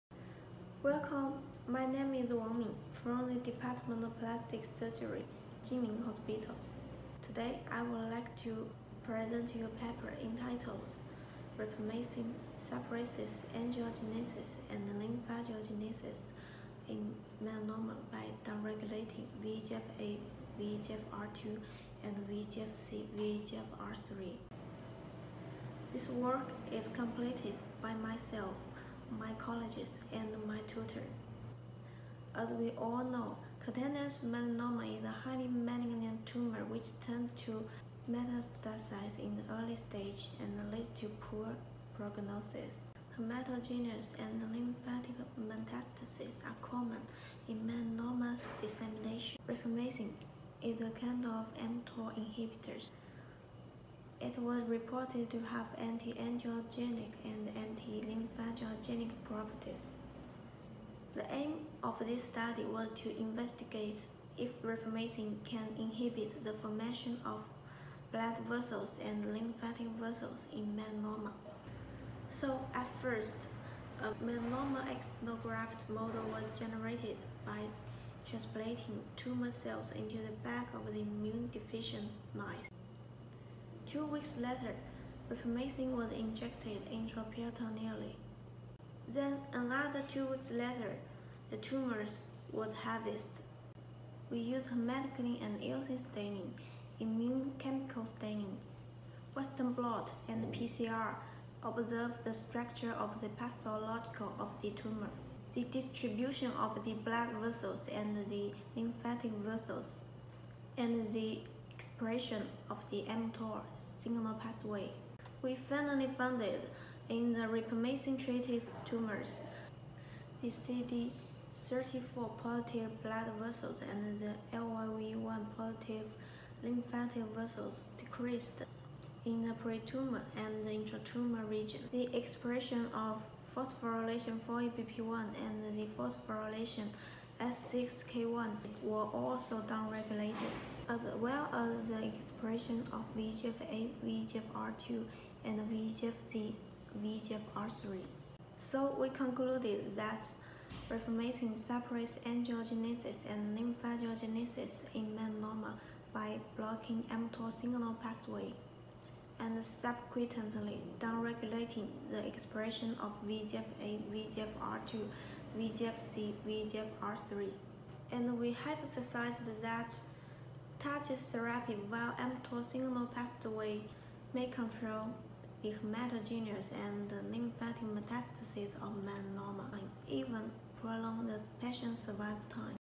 Rapamycin suppresses angiogenesis and lymphangiogenesis in melanoma - Video abstract [ID 205160]
Video abstract of an original research paper &ldquo;Rapamycin suppresses angiogenesis and
lymphangiogenesis in melanoma by downregulating VEGF-A/VEGFR-2 and VEGF-C/VEGFR-3 expression&rdquo; published in the open access journal OncoTargets and Therapy by Wang M, Xu Y, Wen GZ, et al.

Background: Cutaneous melanoma is a highly malignant tumor which tends to metastasize in the early stage and leads to poor prognosis. Hematogenous and lymphatic metastasis are common in the dissemination of melanoma. Rapamycin, an mTOR inhibitor, was reported to have anti-angiogenic and anti-lymphangiogenic properties.
Aim: The aim of this study was to investigate if rapamycin can inhibit the formation of blood vessels and lymphatic vessels in melanoma.
Methods: A melanoma xenograft model was generated by subcutaneously transplanting A375 human melanoma cells into the back of immunodeficient mice. Two weeks after cell transplantation, rapamycin was injected intraperitoneally every other day seven times. Then, tumors were harvested.Hematoxylin-eosin (H-E)staining, immunohistochemical staining, Western blot, and quantitative PCR were performed to observe the pathological structure of the tumor, the distribution of blood vessels and lymphatic vessels, and the expression of mTOR signal pathway, VEGF-A/VEGFR-2, and VEGF-C/VEGFR-3.
Results: The results showed that CD34(+) blood vessels and LYVE-1(+) lymphatic vessels decreased in the peritumor and intratumor region in rapamycin-treated tumors. Expression of p-4EBP1 and p-S6K1 proteins was downregulated. Expression of both proteins and mRNAs of VEGF-A/VEGFR-2 and VEGF-C/VEGFR-3 was downregulated.
Conclusion: In conclusion, rapamycin suppresses angiogenesis and lymphangiogenesis in melanoma by blocking the mTOR signal pathway and subsequently downregulating the expression of VEGF-A/VEGFR-2 and VEGF-C/VEGFR-3. Therefore, targeted therapy via mTOR signal pathway may control the hematogenous and lymphatic metastasis of melanoma, and even prolong patients&rsquo; survival time.